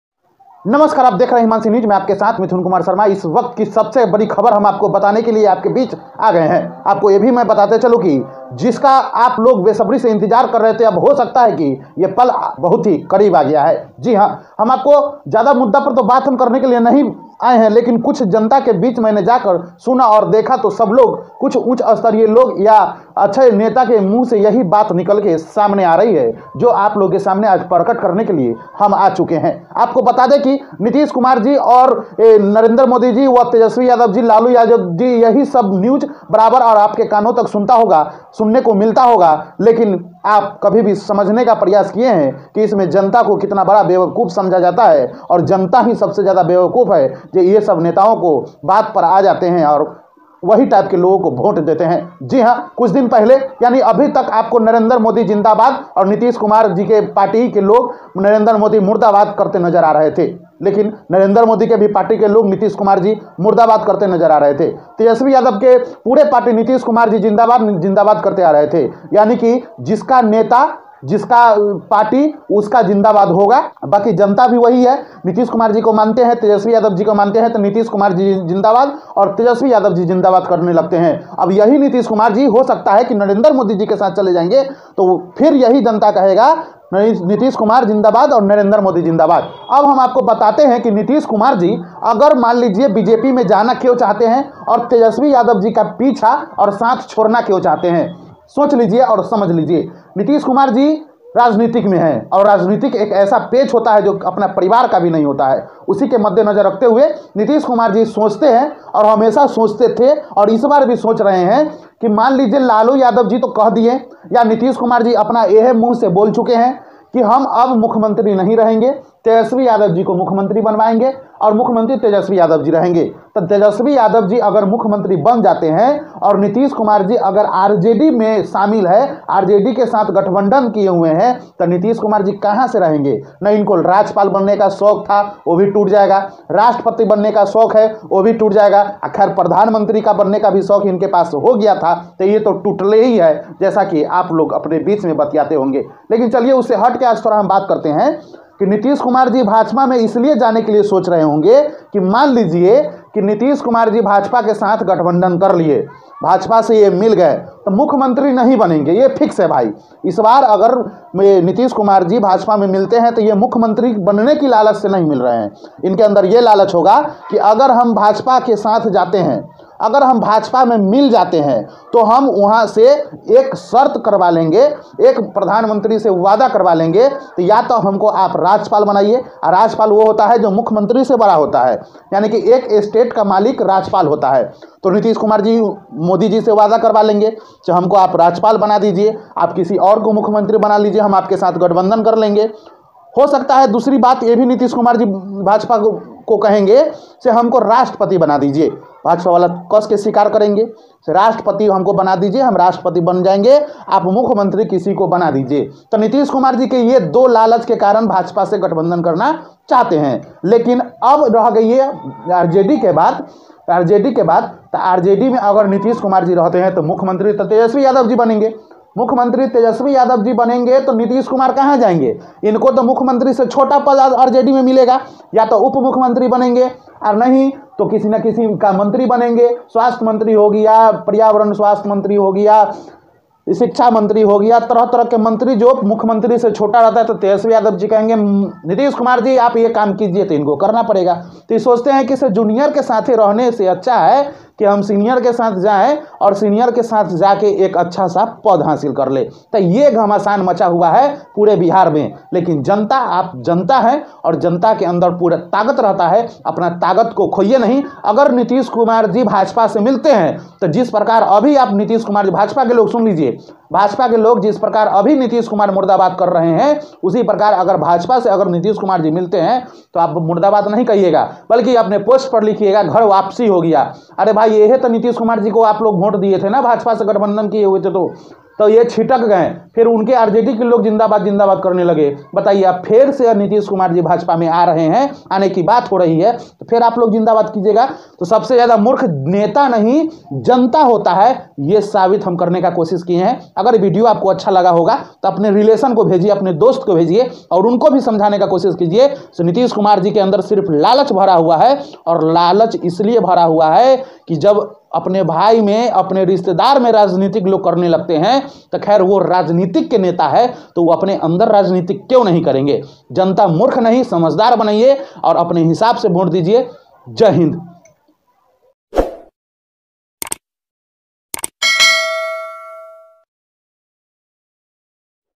जनता को मूर्ख समझने वाले नेताओं का विचार जान लीजिए !
जनता को मूर्ख समझने वाले नेताओं का विचार जान लीजिए ! आखिर नीतीश कुमार पुन: दल बदलू क्यों हो रहे हैं ! नीतीश कुमार राजपाल व राष्ट्रपति का सपना देख रहे !

MUZAFFARPUR LIVE Muzaffarpur Now Nitish Kumar Tejashwi Yadav BJP Bihar Manish Kasyap Sach Talks Amit Shah Narendra Modi सबके राम